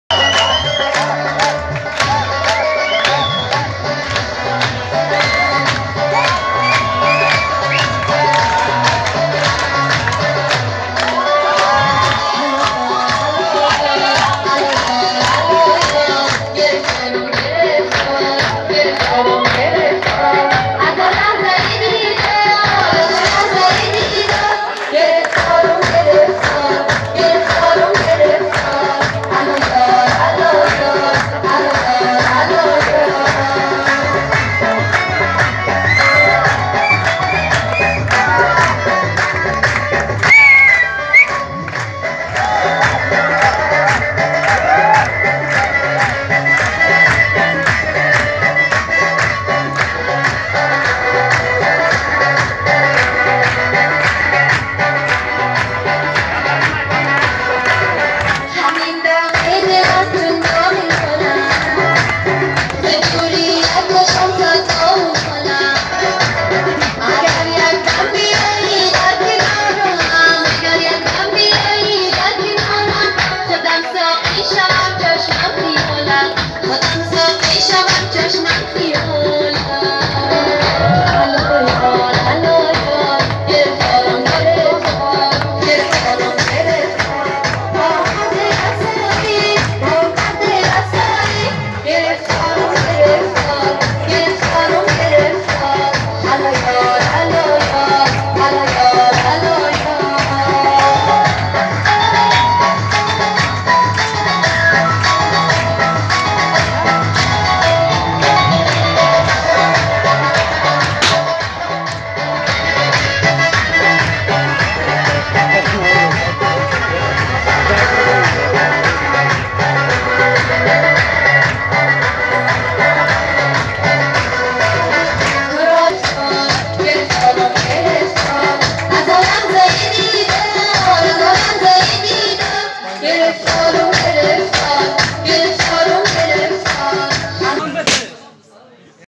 Afghan Futsal Team Championship with dance ❤ ⚽️
تجلیل از نایب قهرمانی تیم فوتسال افغانستان با رقص و پایکوبی ما خجسته باد ❤ ⚽️ تل دی وی افغانستان